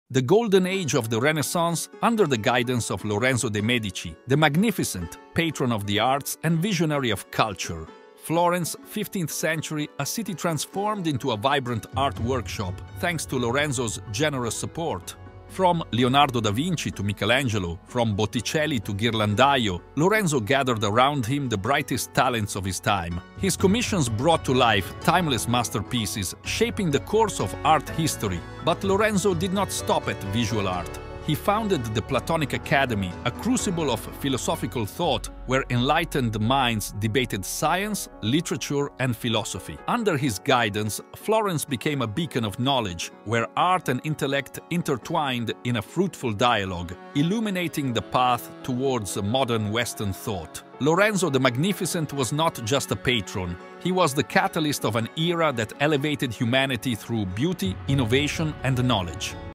Lorenzo the Magnificent
Italian art and culture owe a great deal to one of its most important patrons: Lorenzo de' Medici.